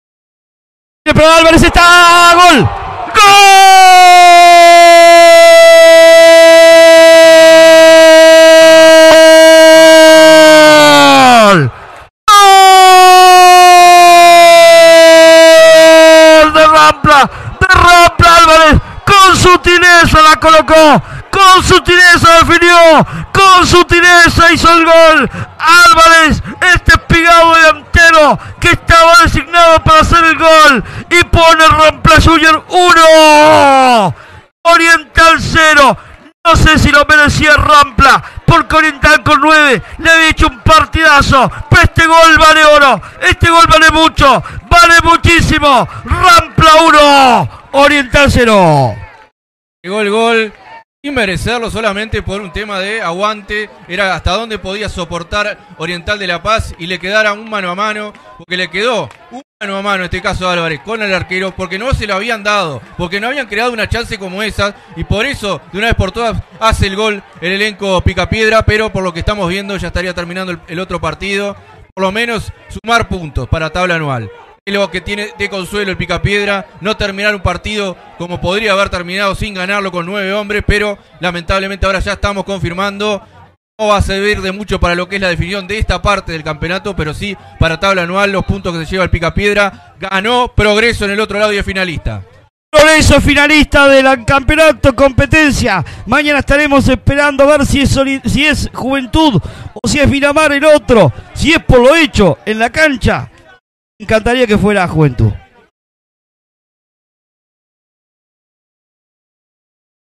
Rampla 1 Oriental 0 - Segunda 2023 (por Gustavo Rodríguez
El gol del partido, por Gustavo Rodríguez y Rafael Pozzi para @losdeafueranosondepalo